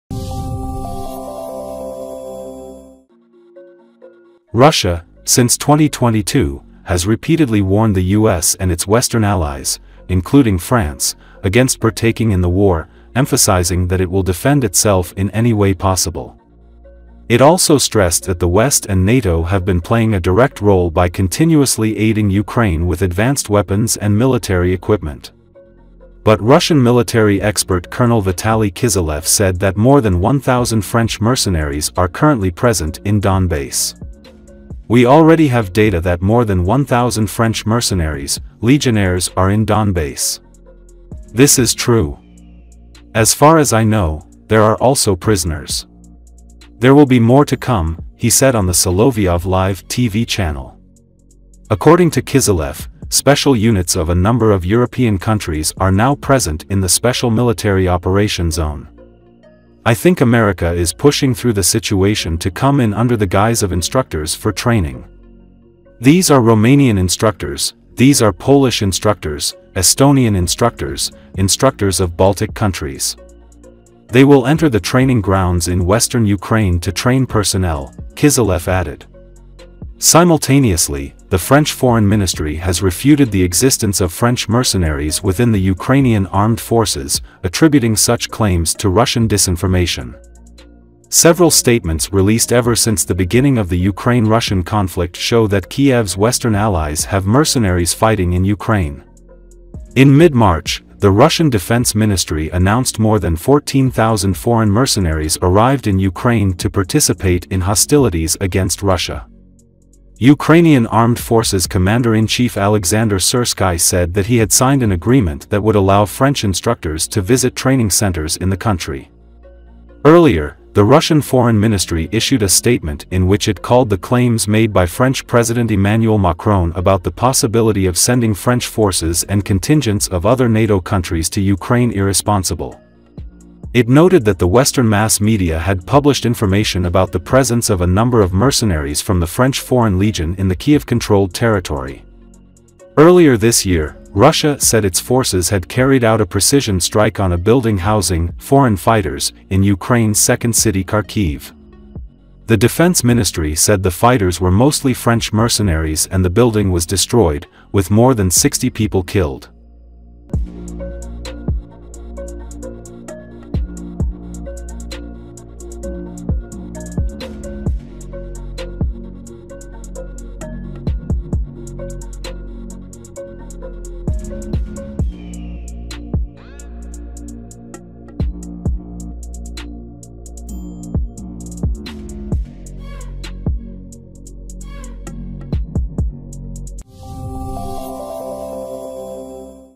More than 1000 French legionnaires are fighting against Russians in Donbass - expert
Russia, since 2022, has repeatedly warned the US and its Western allies, including France, against partaking in the war, emphasizing that it will defend itself in any way possible. It also stressed that the West and NATO have been playing a direct role by continuously aiding Ukraine with advanced weapons and military equipment.
But Russian military expert colonel Vitaly Kiselev said that more than 1,000 French mercenaries are currently present in Donbass.
"We already have data that more than 1,000 French mercenaries, legionnaires are in Donbass. This is true. As far as I know, there are also prisoners. There will be more to come," he said on the Solovyov Live TV channel.
According to Kiselev, special units of a number of European countries are now present in the special military operation zone.
"I think America is pushing through the situation to come in under the guise of instructors for training. These are Romanian instructors, these are Polish instructors, Estonian instructors, instructors of Baltic countries. They will enter the training grounds in western Ukraine to train personnel," Kiselev added.
Simultaneously, the French Foreign Ministry has refuted the existence of French mercenaries within the Ukrainian Armed Forces, attributing such claims to Russian disinformation. Several statements released ever since the beginning of the Ukraine-Russian conflict show that Kiev's Western allies have mercenaries fighting in Ukraine.
In mid-March, the Russian Defense Ministry announced more than 14,000 foreign mercenaries arrived in Ukraine to participate in hostilities against Russia.
Ukrainian armed forces Commander-in-Chief Alexander Sirsky said that he had signed an agreement that would allow French instructors to visit training centers in the country. Earlier, the Russian Foreign Ministry issued a statement in which it called the claims made by French President Emmanuel Macron about the possibility of sending French forces and contingents of other NATO countries to Ukraine irresponsible. It noted that the Western mass media had published information about the presence of a number of mercenaries from the French Foreign Legion in the Kiev-controlled territory.
Earlier this year, Russia said its forces had carried out a precision strike  on a building housing "foreign fighters" in Ukraine's second city Kharkiv. The defence ministry said the fighters were mostly French mercenaries and the building was destroyed, with more than 60 people killed.